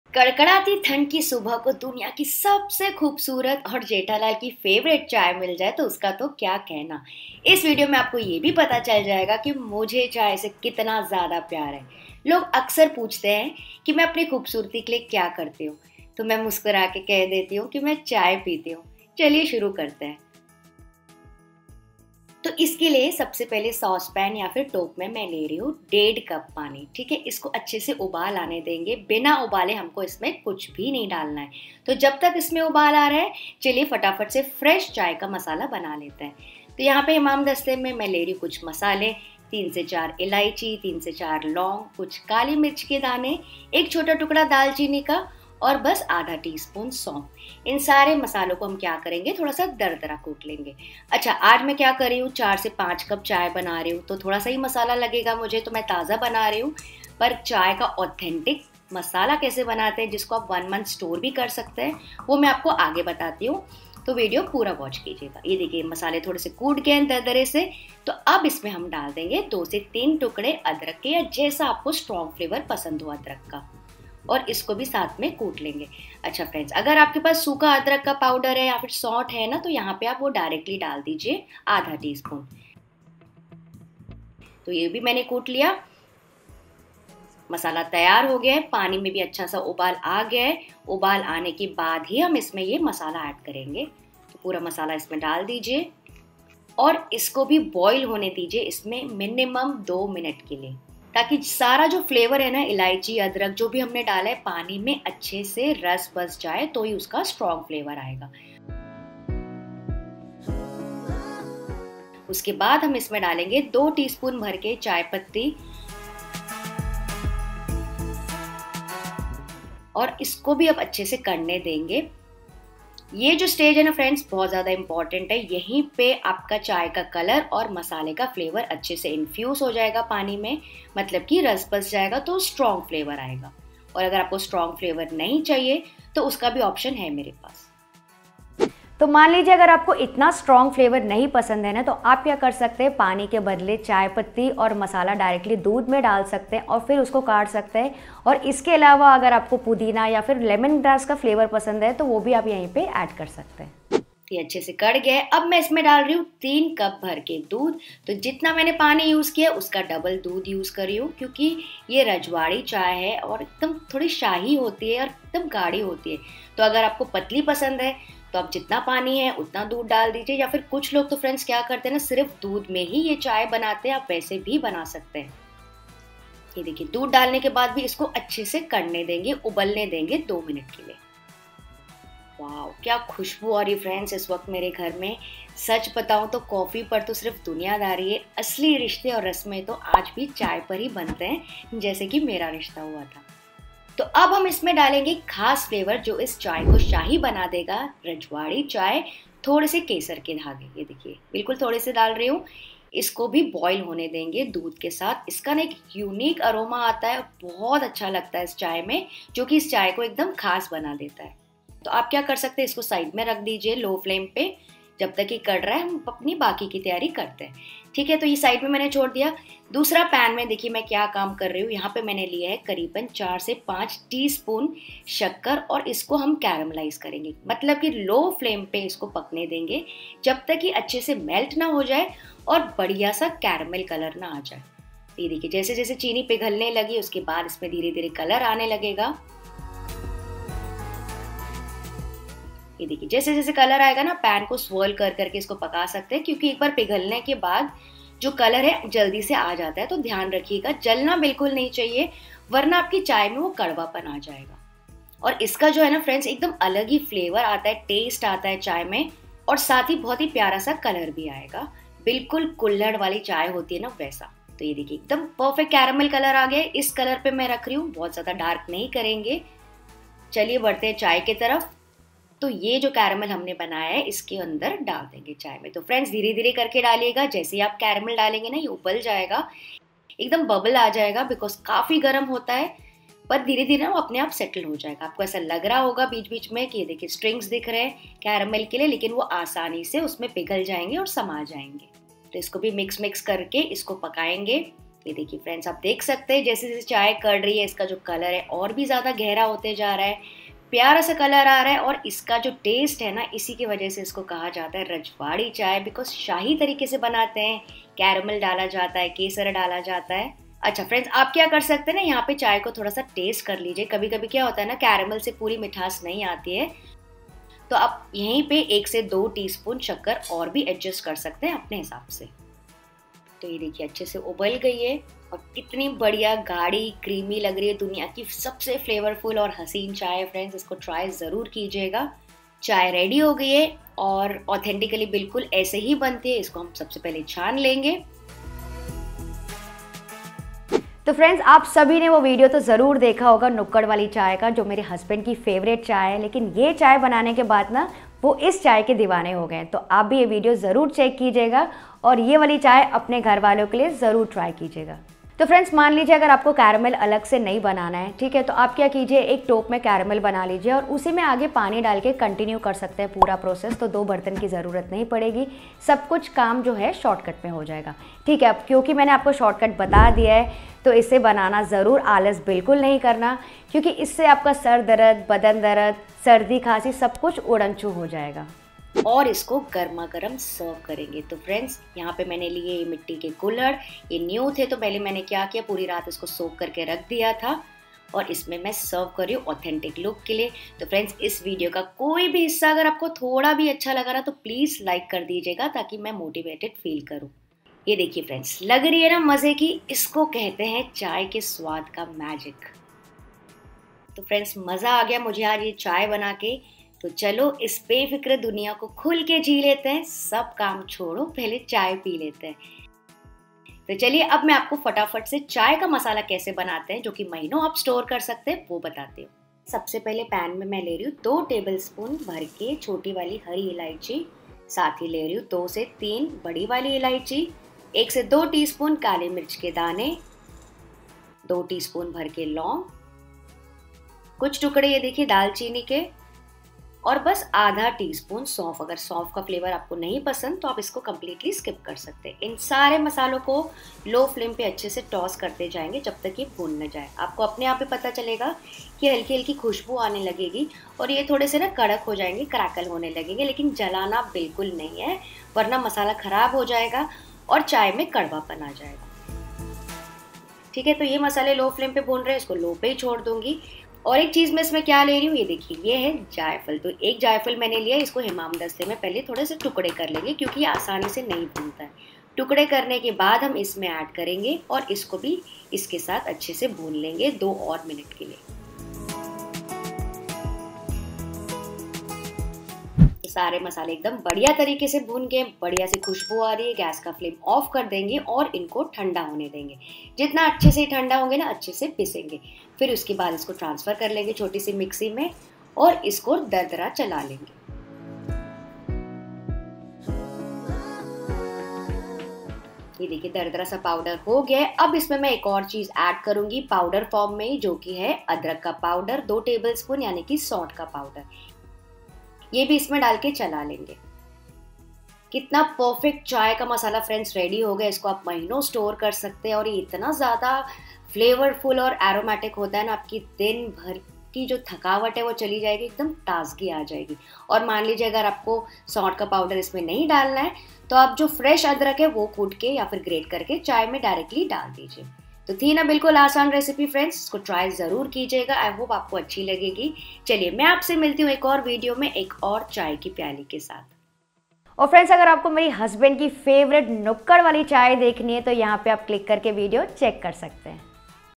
सर्दियों के लिए स्पेशल शाही रजवाड़ी चाय बनाने की आसान और सीक्रेट रेसिपी | Rajasthani Rajwadi Chai
Rajwadi Chai Recipe:
Aaj hum banaenge ek khaas aur swadisht chai - Rajwadi Chai. Ismein hai ek mishran of elaichi, dalchini, aur adrak, jo dega aapko ek shahi anubhav. Chaliye, chai ki pyaali uthaiye aur swaad se bharpoor kariye!

🔔 New videos coming up every Mon, Wed and Sat 2pm

▬▬▬ 📗 Ingredients 📗 ▬▬▬
1.5 cup water
3-4 pcs elaichi
3-4 laung
Some black pepper seeds
1 dalchini
1/2 tsp saunf
2-3 pcs of ginger
2 tsp chai patti
3 cups milk
Some kesar strands
4-5 tsp sugar

▬▬▬📗My Recipe Book📗▬▬▬